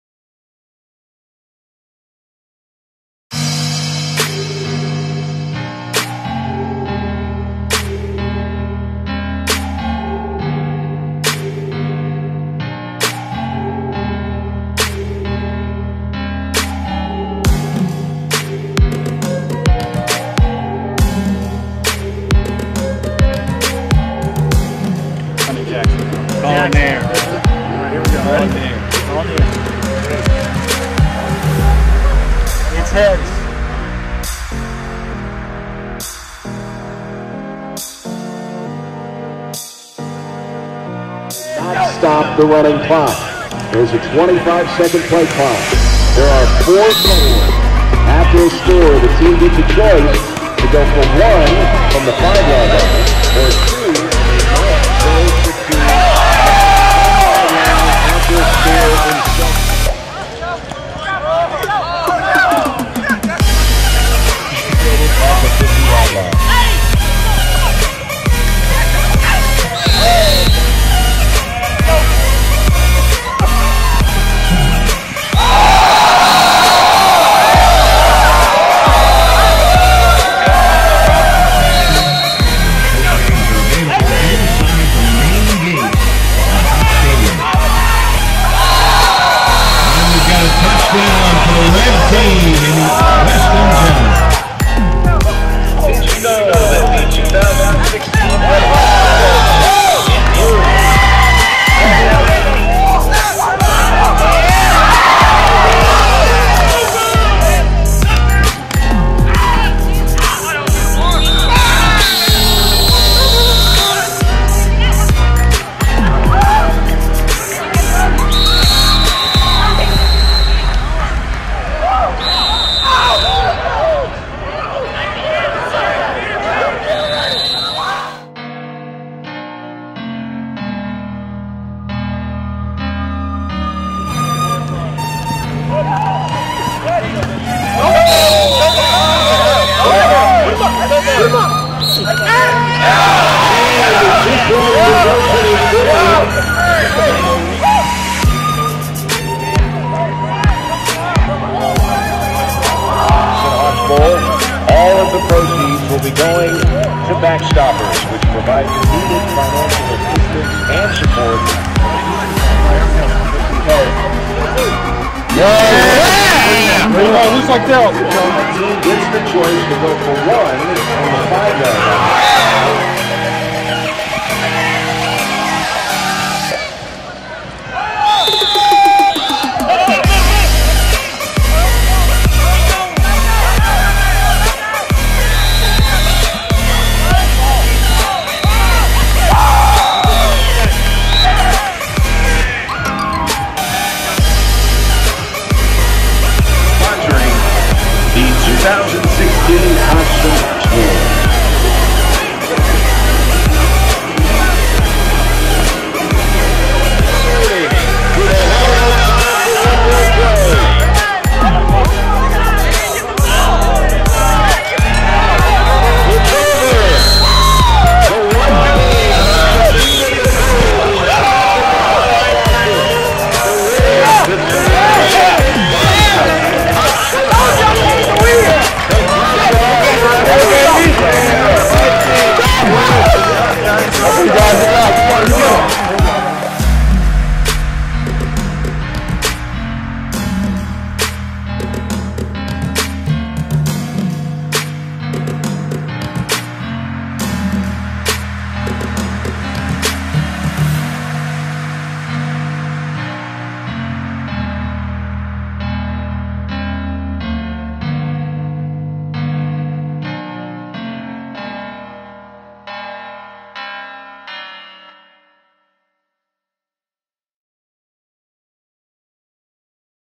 2016 Cops & Hawks Bowl
The first ever Cops & Hawks Bowl. A flag football game held to raise money to benefit Backstoppers.